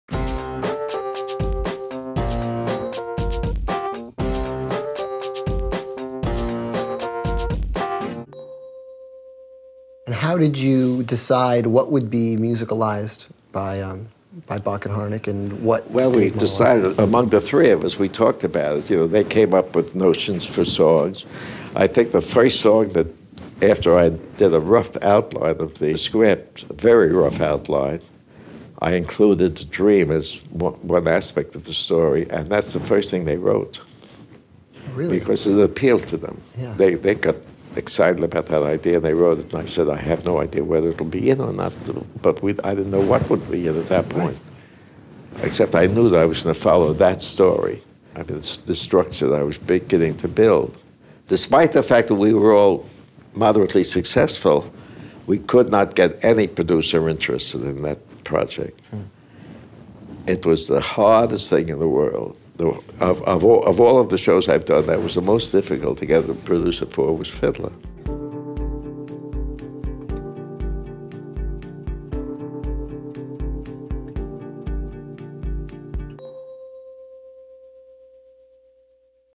Joseph Stein & Lin-Manuel Miranda: Artist Collaboration
"Fiddler on the Roof" and "Zorba" playwright Joseph Stein talks with a pre-Hamilton Lin-Manuel Miranda on how the book and songs of a musical come together in The Legacy Project Volume I.

Produced by the Dramatists Guild Foundation, the Legacy Project is a series of interviews featuring prominent contemporary American playwrights, lyricists and composers. Each interview offers an intimate look into the lives and creative process of these writers of the theater.